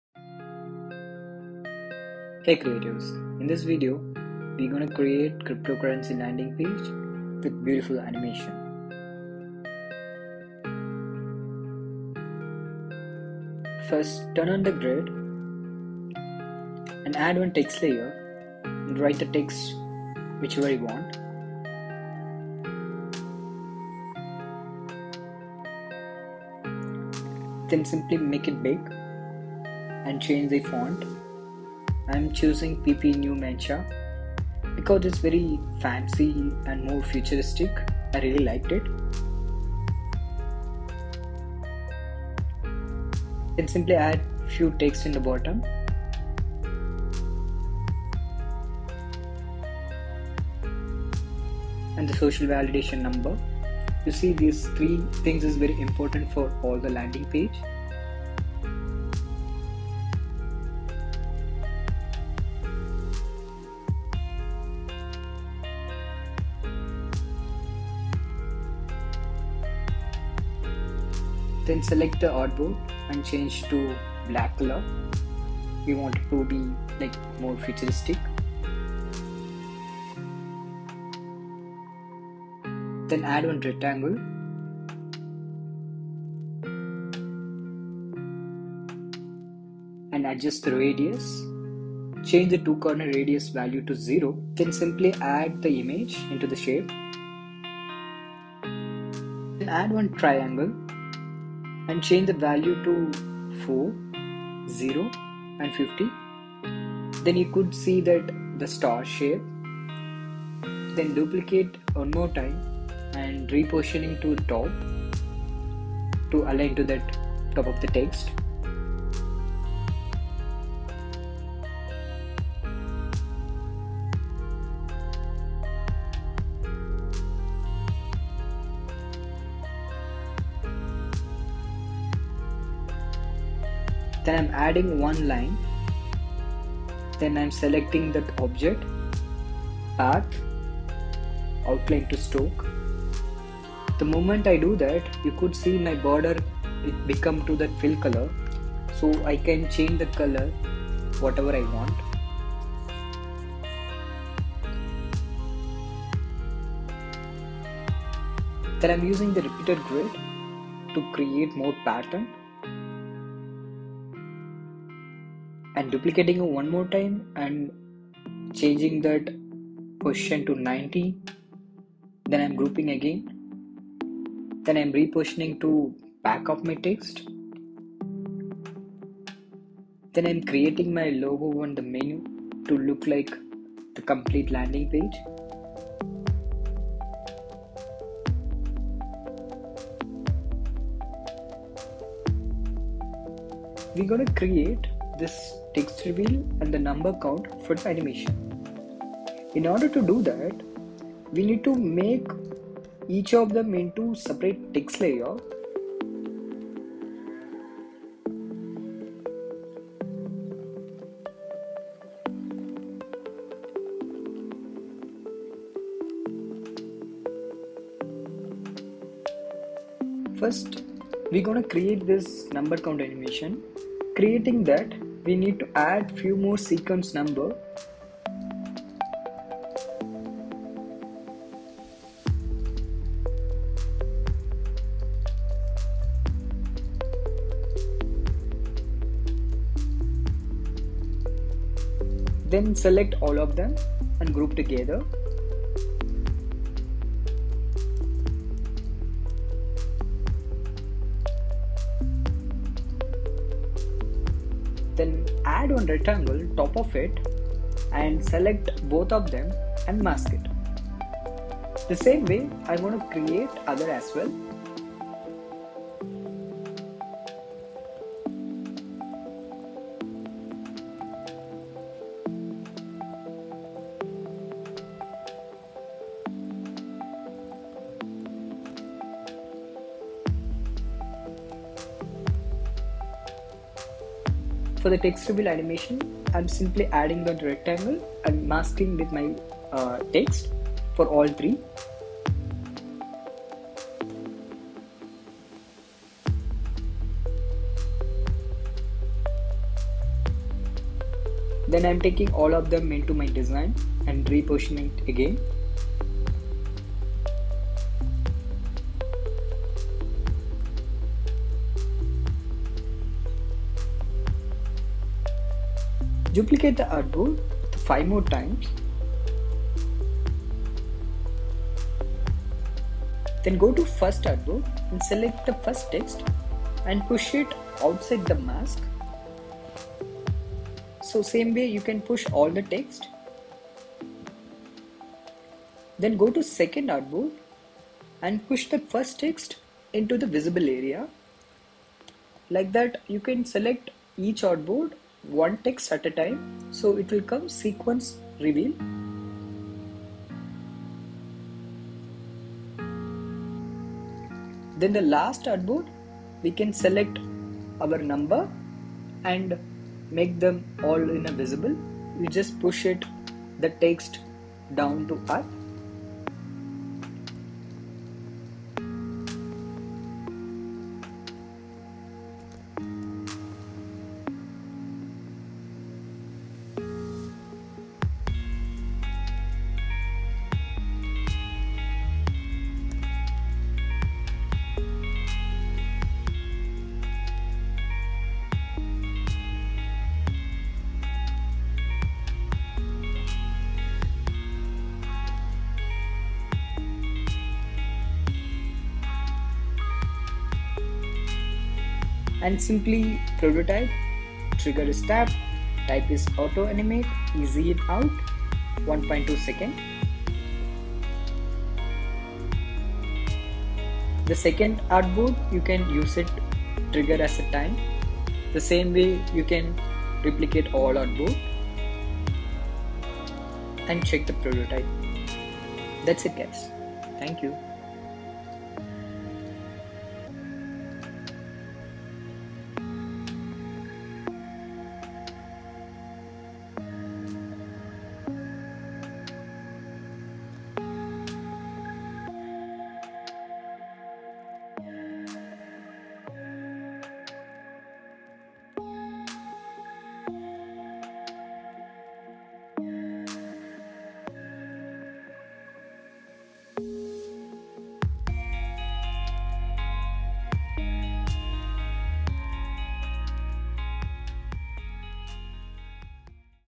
Interactive crypto Landing page
Hello Everyone.
You can learn how to design simple and creative landing pages using simple techniques!

❤️ learn design with me.